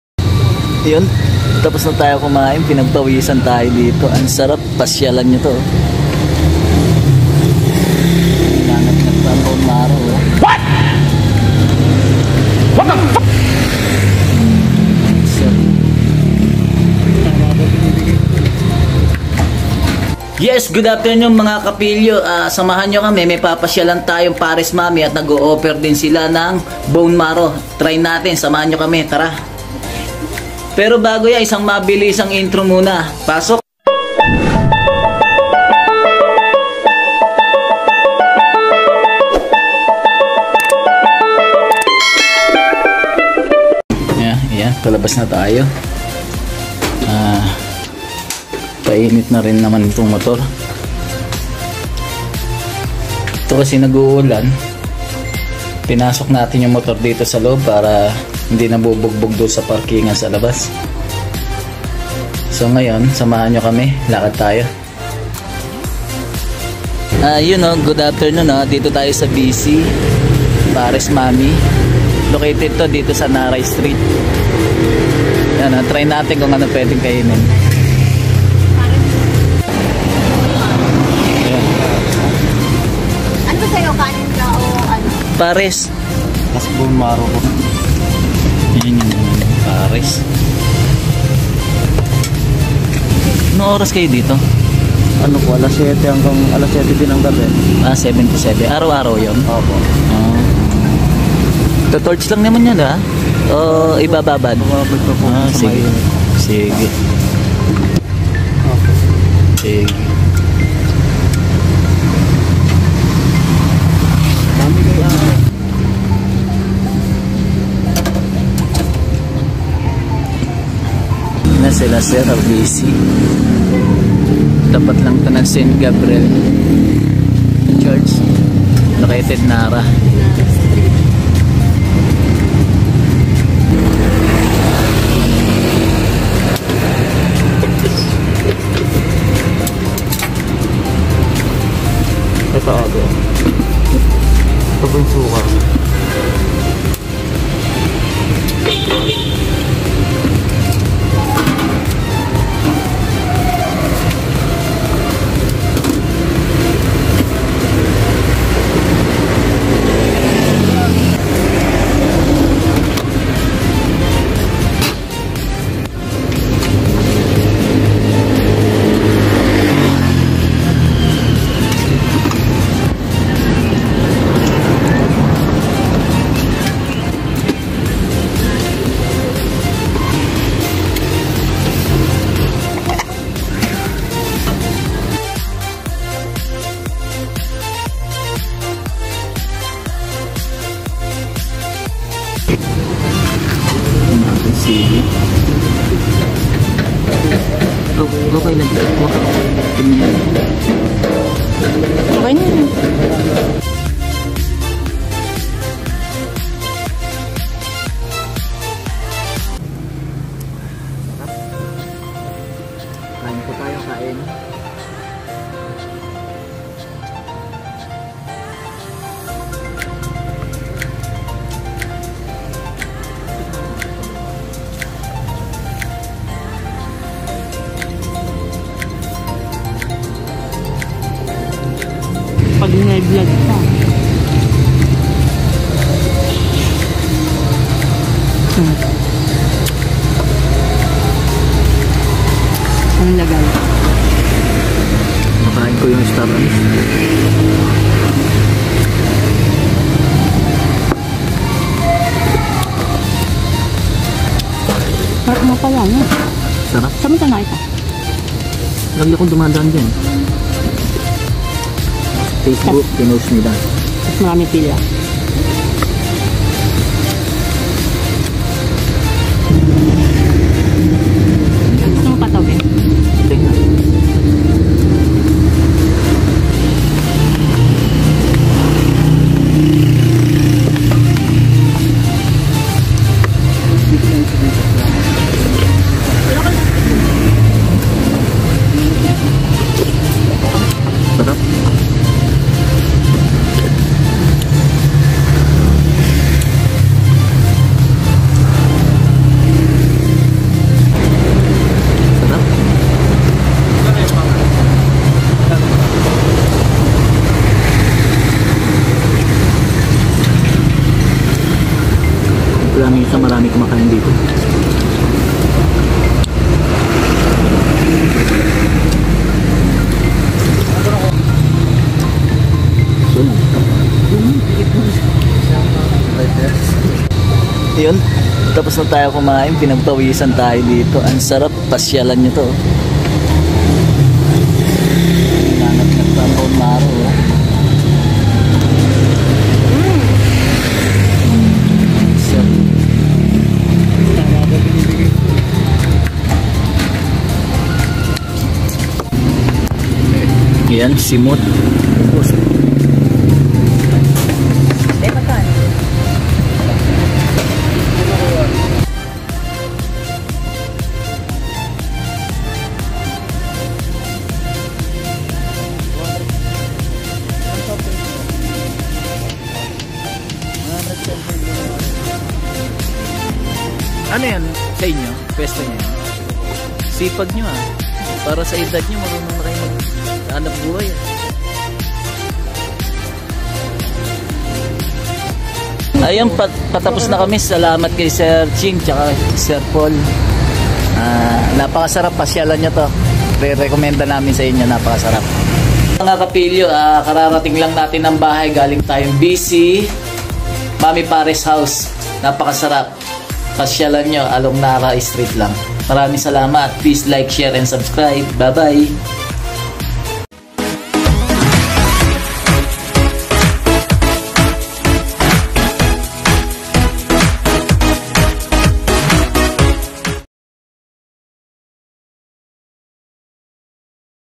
Putok Batok PARES with BONE MARROW /V'C MAMI PARES HOUSE at Narra st.Marikina City
Isang uri ng simpleng sabaw na sinasahugan ng karne ng baka at iba pang sangkap ..
Na ang kapares nito kapag kinain ay pwedeng mami noodles o kaya ay fried rice ..